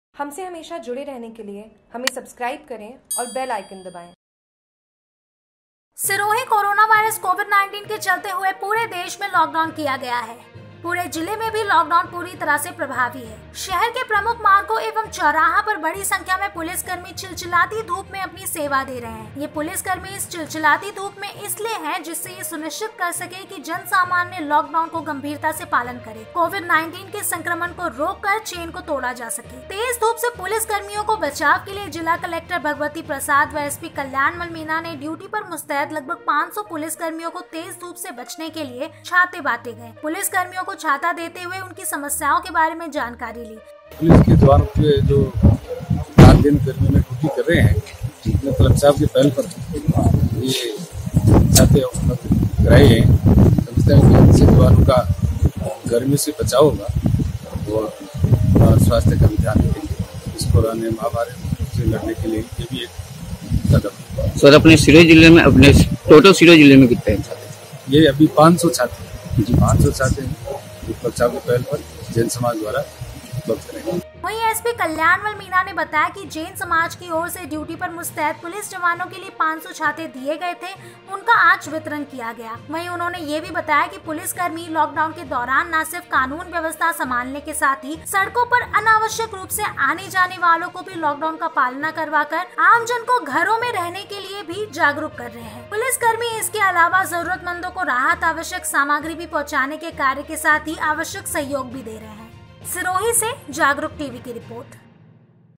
Jagruk TV सिरोही कोरोना वायरस कोविड.19 के चलते हुए पूरे देश में लाकडाउन किया गया है
Jagruk TV

सिरोही कोरोना वायरस कोविड.19 के चलते हुए पूरे देश में लाकडाउन किया गया है। पूरे जिले में भी लाकडाउन पूरी तरह से प्रभावी है। शहर के प्रमुख मार्गो एवं चौराहों पर बड़ी संख्या में पुलिस कर्मी चिलचिलाती धूप में अपनी सेवा दे रहे हैं ये पुलिस कर्मी इस चिलचिलाती धूप में इसलिए है जिससे ये सुनिश्चित कर सके कि जनसामान्य लाक डाउन का गम्भीरता से पालन करे कोविड.19 के संक्रमण को रोक कर चैन को तोड़ा जा सके। तेज धूप से पुलिस कर्मियों को बचाव के लिए जिला कलक्टर भगवती प्रसाद व एसपी कल्याणमल मीना ने ड्युटी पर मुस्तैद लगभग 500 पुलिस कर्मियों को तेज धूप से बचने के लिए छाते बाटे गये।  पुलिस कर्मियों को छाता देते हुए उनकी समस्याओं के बारे में जानकारी ली। वही एसपी कल्याणमल मीना ने बताया की जैन समाज की ओर से ड्युटी पर मुस्तैद पुलिस जवानों के लिए 500 छाते दिए गए थे उनका आज वितरण किया गया। वही उन्हाने यह भी बताया पुलिसकर्मी लाकडाउन के दौरान न सिर्फ कानून व्यवस्था सभालने के साथ ही सड़को पर अनावश्यक रूप से आने-जाने वालो को भी लाकडाउन का पालन करवाकर आमजन को घरों में रहने के लिए भी जागरूक कर रहे है। पुलिस कर्मी इसके अलावा जरूरतमंदों को राहत आवश्यक सामग्री भी पहुचाने का कार्य के साथ ही आवश्यक सहयोग भी दे रहे है।